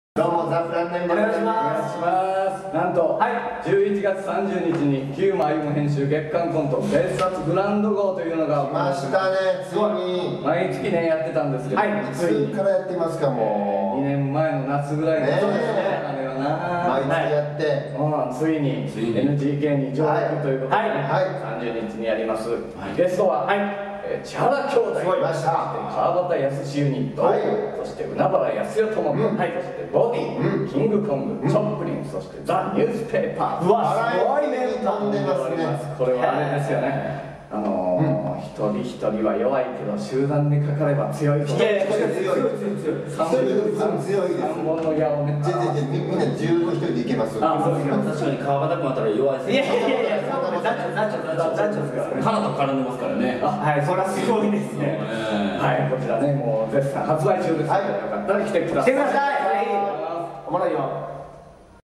11/30(水) NGK公演「久馬歩編集 月刊コント 別冊グランド号」開催決定！
久馬歩編集 月刊コント 別冊グランド号

おかげさまで、なんばグランド花月は今年の１１月で２４周年を迎えます。
それを記念して、ザ・プラン９のお～い！久馬編集による人気イベント「月刊コント」が、スペシャル版として なんばグランド花月に『別冊グランド号』として初上陸します！
今回は、なんといっても千原兄弟が、月刊コントに初参戦。また、吉本新喜劇から川畑座長が、すっちー、松浦真也など個性的な座員を引き連れて登場。また普段は漫才のイメージが強いキングコングがなんとコントを披露してくれます。そのほか、海原やすよ ともこ、ＢＯＤＹ（増谷キートン、椿鬼奴）、など華やかで個性的なメンバーが集まりました。
気になる外部ゲストですが、あの「社会風刺コント集団」ザ・ニュースペーパーから松下アキラ・福本ヒデ、そして一味も二味も違ったコントを見せてくれるチョップリンが出演決定しました。
さぁ、久馬編集長がこの出演メンバーのコントをどうやって一つの芝居にまとめあげるのか!?　見どころ満載の『久馬歩編集 月刊コント 別冊グランド号』は、チケット絶賛発売中です！